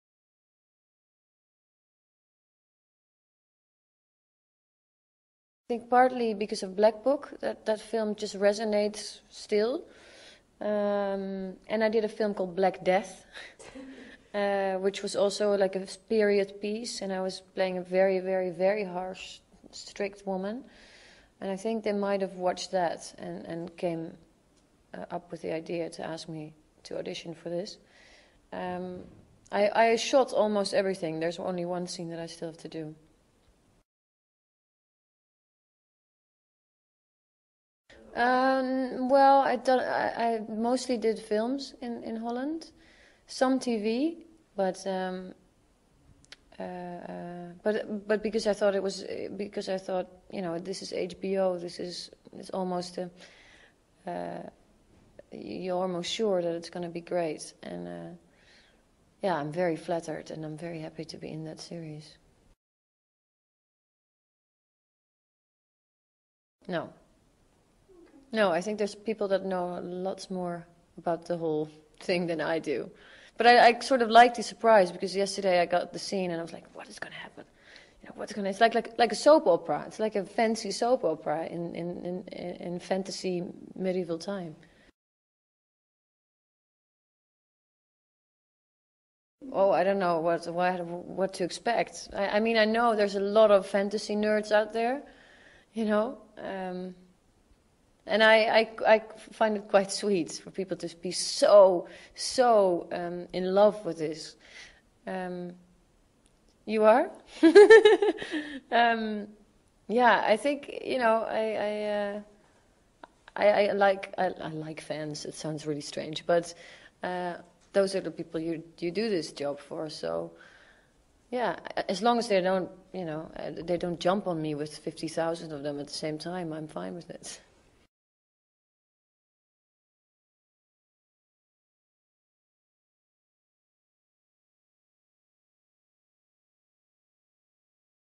Carice Van Houten fala sobre Game Of Thrones
Entrevista com Carice Van Houten (Melisandre) sobre a segunda temporada de Game Of Thrones.
Legendado em pt-br.

Para saber mais visite o Game Of Thrones BR game of thrones 3